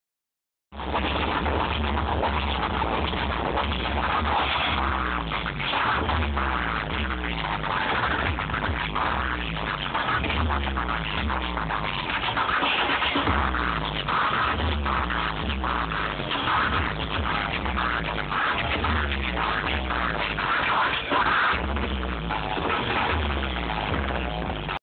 Sid Wilson live at the Rockstar Energy Drink Metal Mayhem Festival
i was at the Rockstar Energy Drink Metal Mayhem Festival